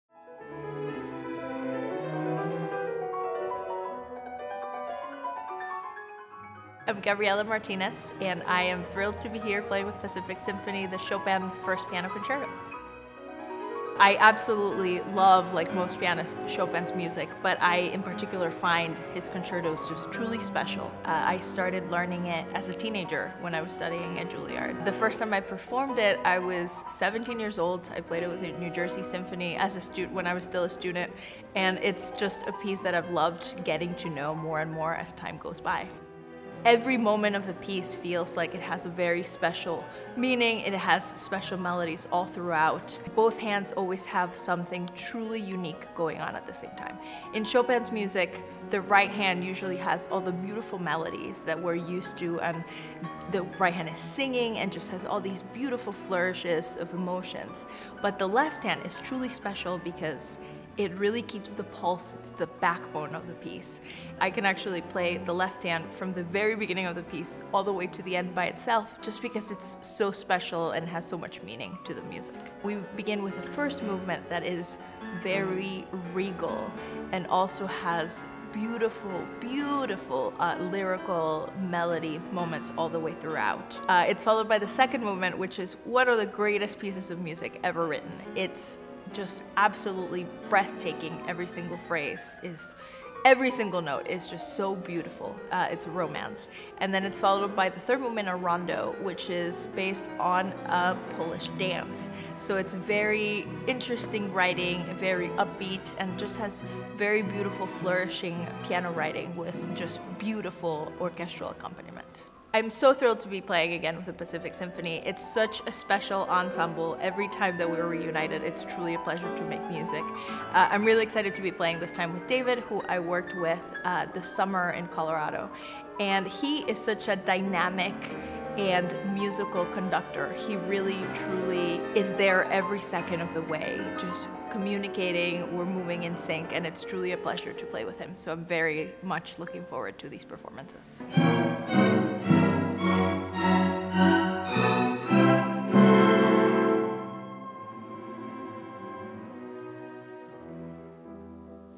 Behind the Scenes - Gabriela Martinez
Genuine, charismatic and down to earth. Gabriela Martinez puts a smile on your face when you talk with her and grabs your attention when she plays. A truly gifted artist that the symphony is honored to play with this weekend.